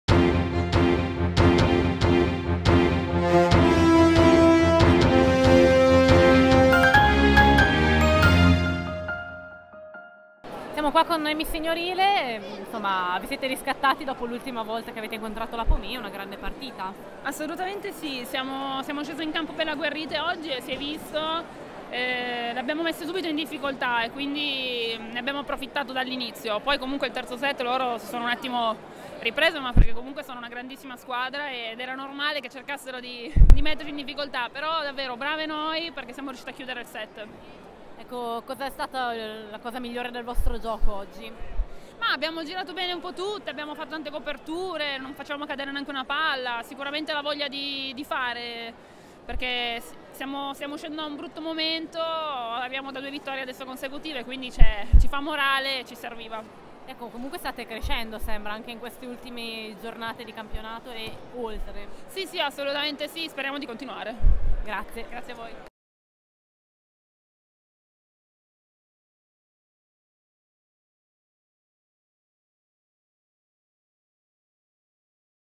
A tu per tu con Noemi Signorile - Unet Yamamay Busto Arisizio
Intervista con Noemi Signorile al termine della semifinale di andata Cev Cup 2017 Pomi Casalmaggiore vs Unet Yamamay Busto Arsizio